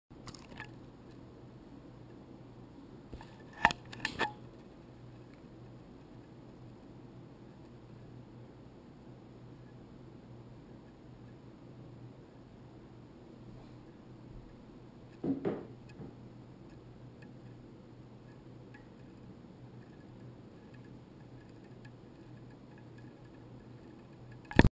GoPro Xtreme - Hanging from the Ceiling Fan Chain
I hung the GoPro from the ceiling fan chain and pulled the chain to turn the light on.  I wanted to add in a little bit extra so I pushed the GoPro around and it swung around on the chain.